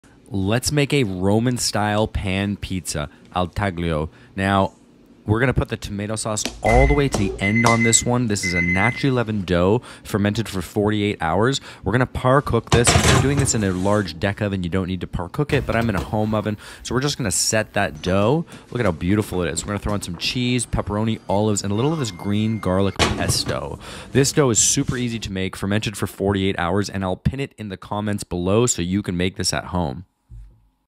Roman Style Pan Pizza
Sorry to the Italians, I know the G is silent but my Canadian accent is thick ;0
It is stretched thinner and cold fermented for 2 days instead of one.